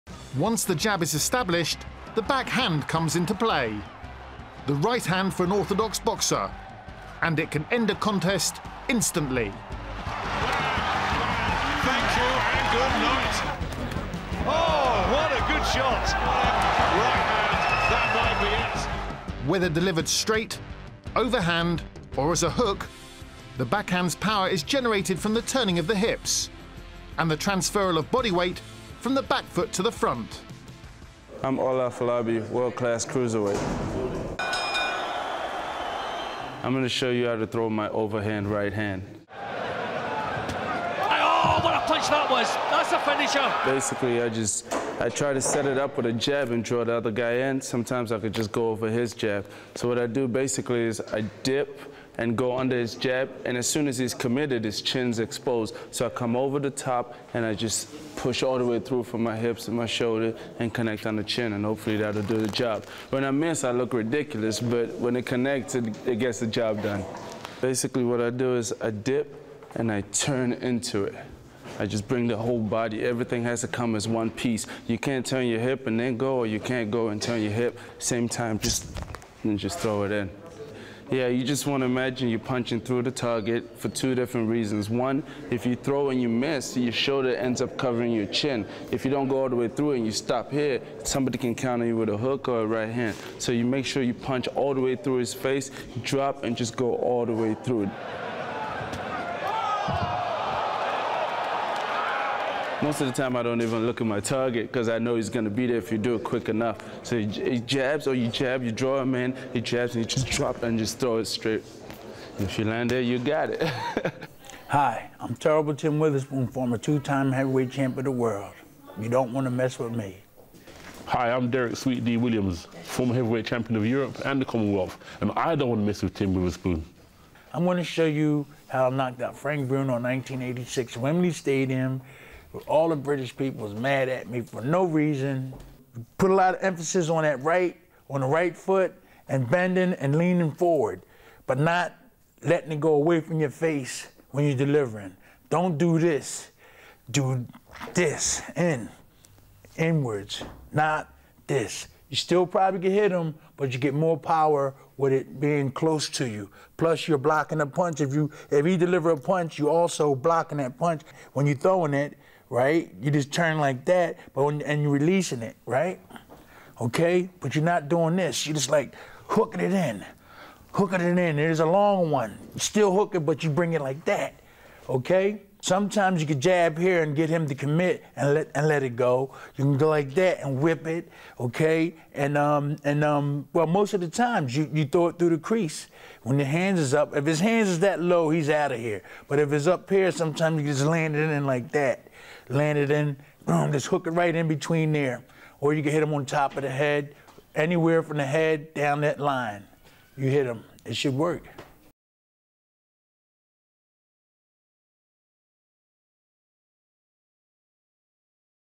How to Throw the Backhand | Ola Afolabi & Tim Witherspoon Masterclass | How To Box
Ola Afolabi and Tim Witherspoon give a lesson in how to throw the backhand.